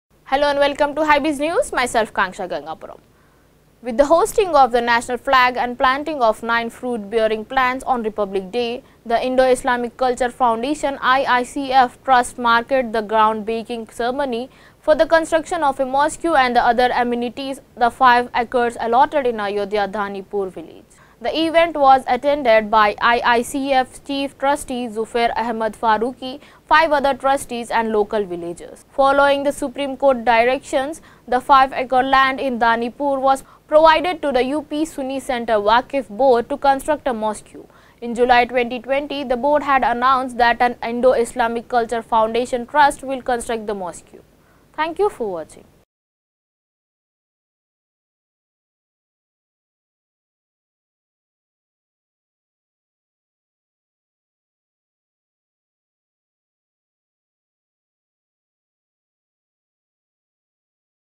Ground-breaking Ceremony of Ayodhya Mosque Held on Republic Day | Hybiz
With the hoisting of the national flag and planting of nine fruit-bearing plants on Republic Day, the Indo-Islamic Cultural Foundation (IICF) Trust marked the groundbreaking ceremony for the construction of a mosque and other amenities on the five acres allotted in Ayodhya’s Dhannipur village.
The event was attended by IICF chief trustee Zufar Ahmad Farooqi, five other trustees, and local villagers.